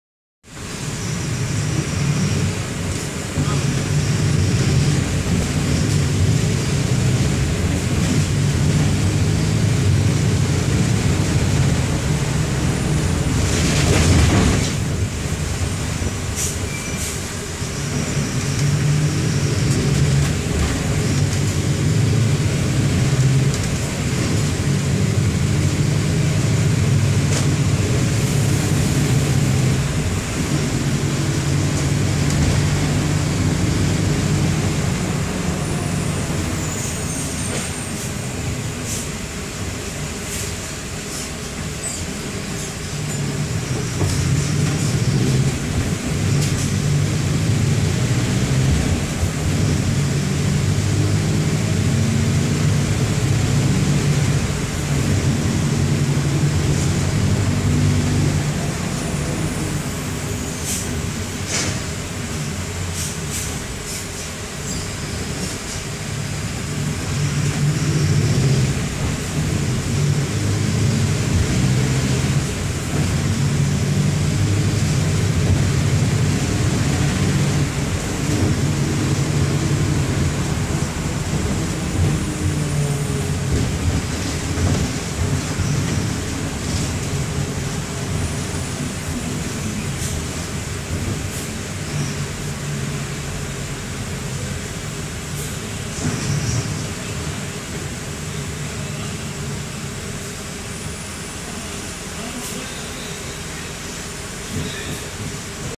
Mascarello Gran Via Midi 2011 Mercedes-Benz OF-1721 BlueTec 5 (Viação Pioneira 224464)
Depois do Svelto 2012 da Pioneira (carro 222984) na linha 255 - Santa Maria 100/200 x Setor M Norte, vamos de Gran Via Midi 2011 da Pioneira (carro 224464) na linha 205.1 - Gama Oeste x Setor M Norte.